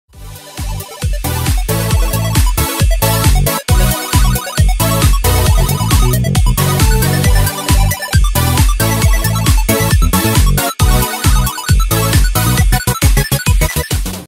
Summer Intro?
Here's another new intro for the summer, animated by yours truly! I hope you guys like it, let me know what you think! Also big thanks to Toni Leys for allowing me to use his music for this and future uploads! But yea, if you guys like this video and wanna see more, you know what to do! PEACE, my NinjaFam, and take care!!!

Links/Credits:
»My Discord Server!!!: